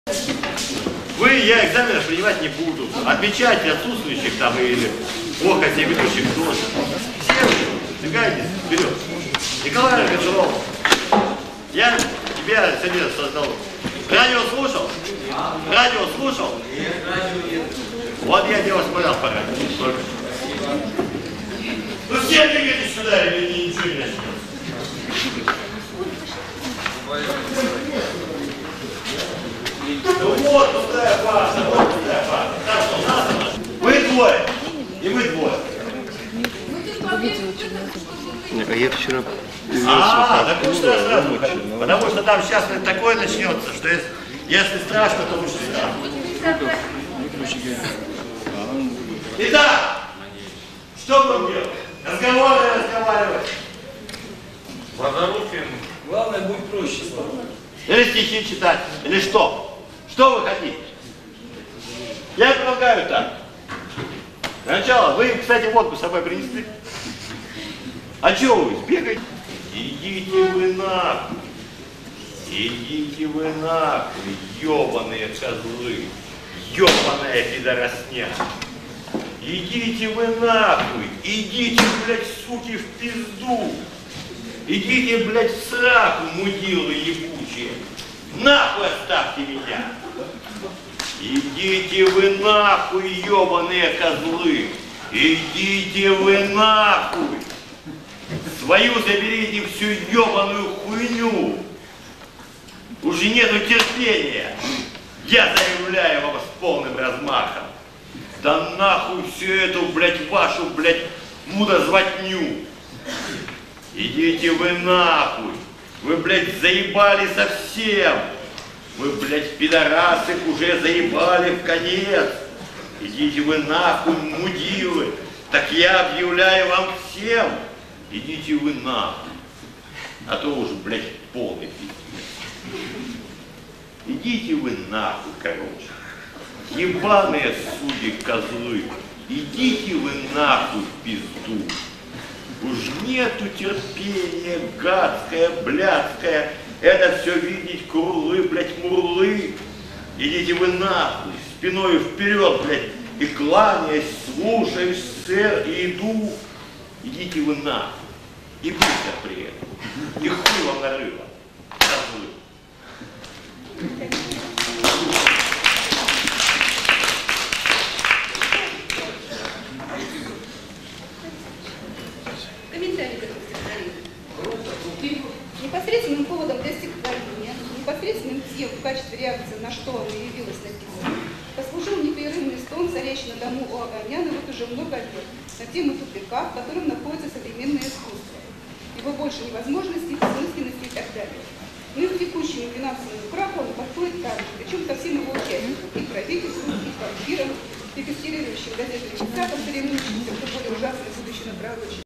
Самый радикальный стих М. Немирова в исполнении автора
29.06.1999 г. Мирослав Немиров выступил в стенах родного Тюменского Университета, где в начале 1980-х не только учился, но частенько и жил, не имея другого дома.